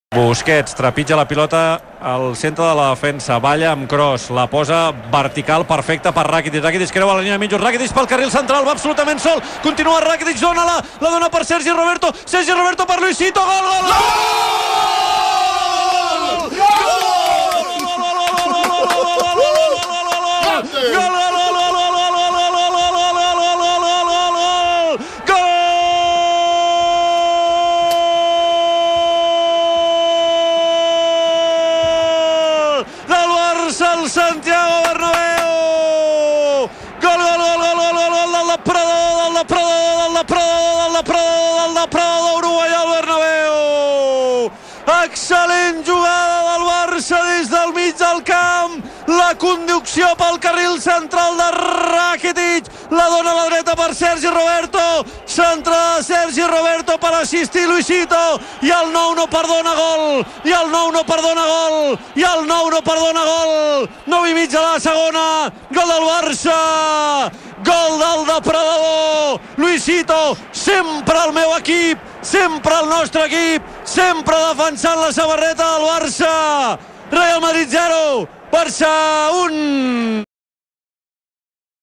El Clàssic 22-D. Gol de Luis Suárez (0-1)
La RAC-Cam d'en Gerard Romero ha gravat els tres momentassos del clàssic. Tres gols mooolt celebrats!